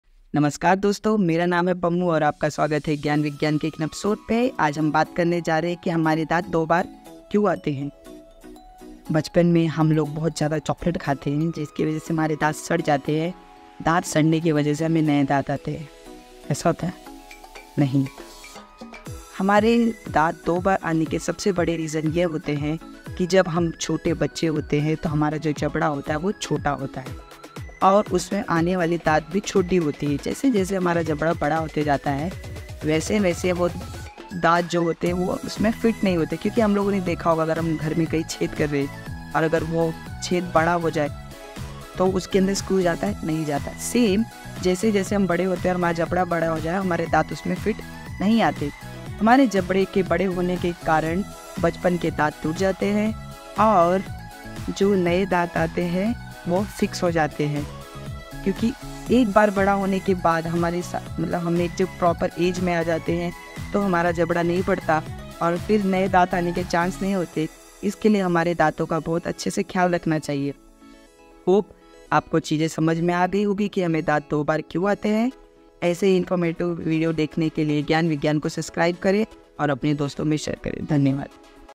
अच्छा तो इसके लिए बचपन के दांत टूट जाते हैं और नए दांत दोबारा आते हैं
बचपन में निकले दांत जिन्हें दूध के दांत कहते हैं छह सात वर्ष के बाद एक एक कर टूटने लगते हैं और नए दांत आने लगते हैं. Baby teeth: मानव शरीर के सभी अंगों की तरह दांत भी अत्यंत महत्वपूर्ण अंग है. सफेद सुंदर दांतों को मोती की उपमा मिलती है. हालांकि दांतों की भूमिका व्यक्तित्व निखारने से लेकर स्वस्थ रहने तक बहुत बड़ी है. मनुष्यों में, दूध के दांत 20 होते हैं। प्राथमिक (शिशु) दांत आमतौर पर 6 महीने की उम्र में आना शुरू हो जाते हैं, और स्थायी दांत आमतौर पर लगभग 6 साल में आने लगते हैं।